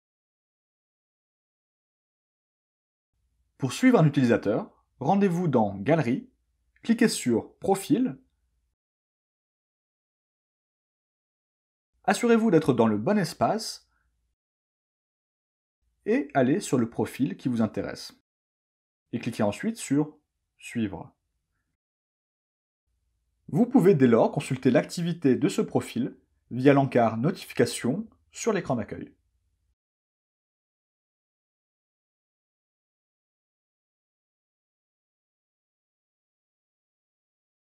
Tuto 12 : Suivre des utilisateurs sur b-connexion (2021)
37 secondes proposées par Victor, notre service civique, pour voir comment suivre des utilistaeurs-trices sur b-connexion.fr, mise à jour de 2021 qui prend en compte la notion d'espaces.

Une vidéo réalisée pour le réseau "B.O.A.T ( Badges ouverts à tous) - Reconnaître en Nouvelle-Aquitaine".